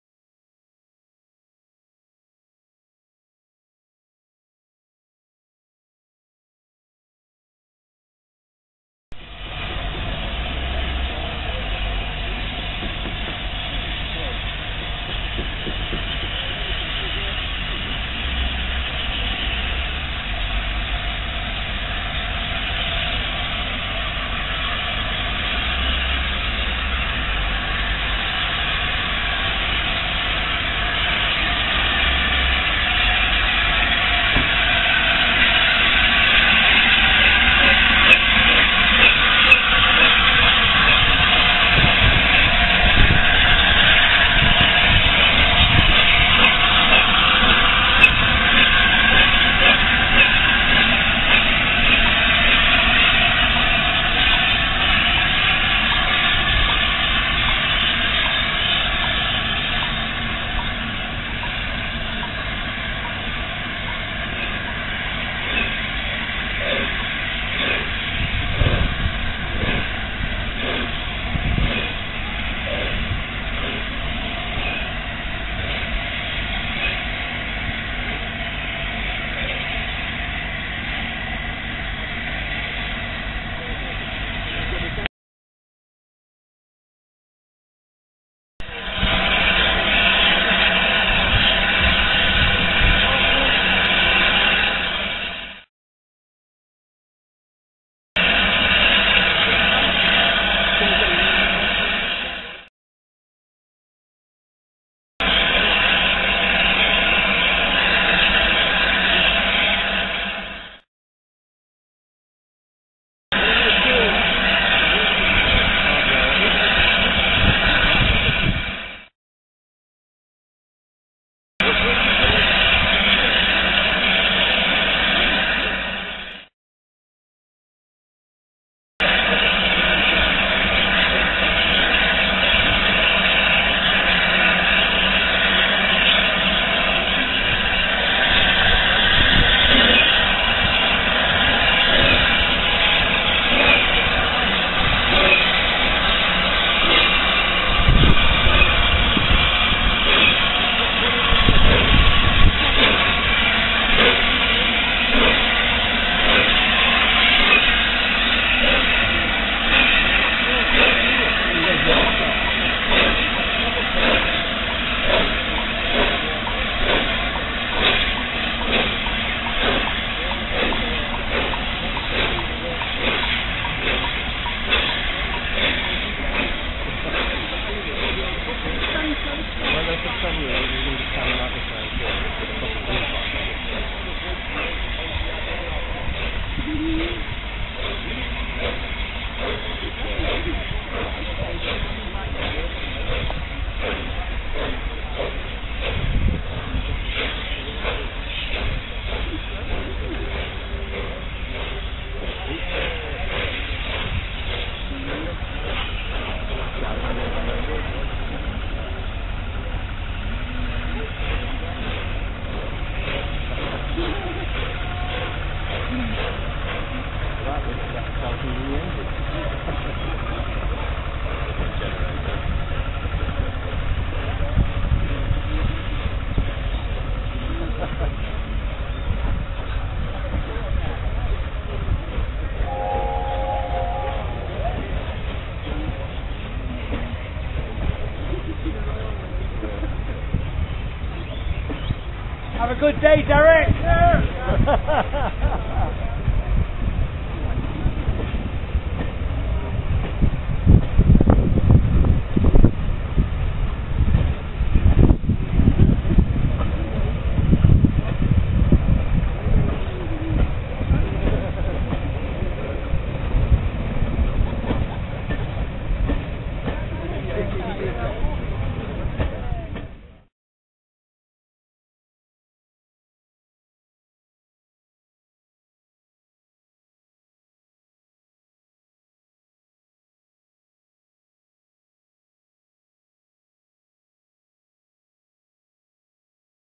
4492 Dominion of New Zealand.wmv
generator vanOn Thursday 7 July 2011 LNER A4 Pacific 4492 "Dominion of New Zealand" arrived in Gloucester station from London via Swindon and Stroud en route to Cardiff.  This locomotive as in fact 60019 "Bittern" in disguise, the BR green livery having been replaced by LNER Garter Blue, the wheels restored to red and the valencing over them replaced.  The purported 4492 also hauled a second tender to increase its water capacity and thereby minimise the stopping needed on the hundreds of miles being covered on the bright summer's day.  As such, it recalled 4472 "Flying Scotsman"  running with a similar second tender in the early 1970s.Electricity for lighting, cooking and other onboard services came from a generator located in a carriage part way down the train.  It will also be noted how gingerly the driver started the heavy train west out of Gloucester.  No slipping at all, compared with Bittern's visit of 2009!